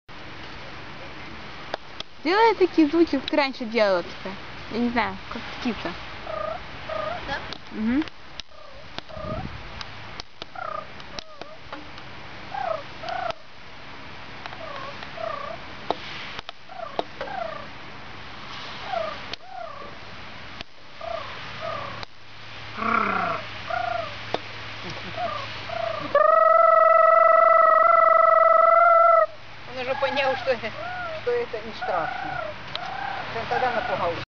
Rabbit Hears Danger
At first, when he heard the sound of the bird, he got really scared. Later, he stopped minding us.